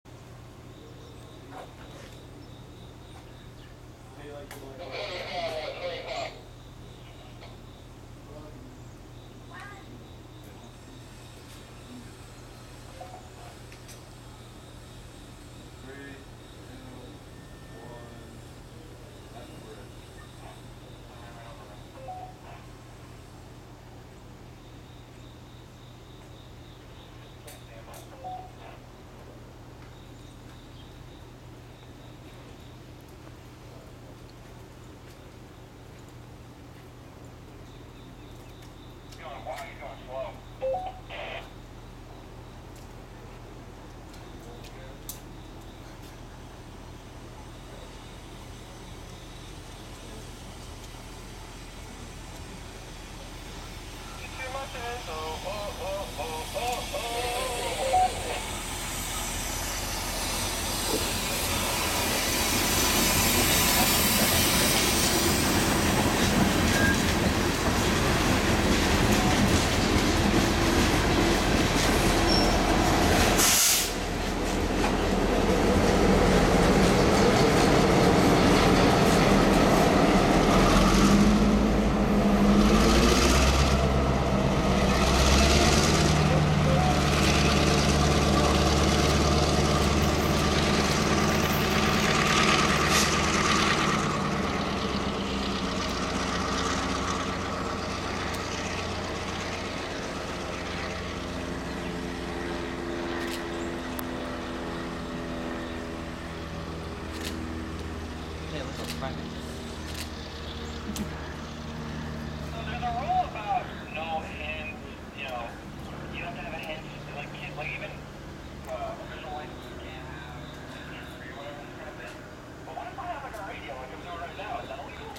Conrail MA-2 Reversing Back Towards Manville Yard With Ex Norfolk & Western and Herzog Car!
Seen at- 11:24am
8/23/2012

Well this ended up being our last train of the long 2 1/2 days at the station. After working the Chemical Co. in Middlesex, CR local MA2 gets permission to reverse back to Manville Yard with its short string of cars. On the way back one of MA2's crew members stands on the back of the last tanker and 5621 shoves the cars as well as the HERZOG ballast car back to the yard to an approach signal. And from this we'd begin the nice walk back to my Grandma's.

CR MA2:
1-NS (Ex N&W #4128) GP38-2 #5621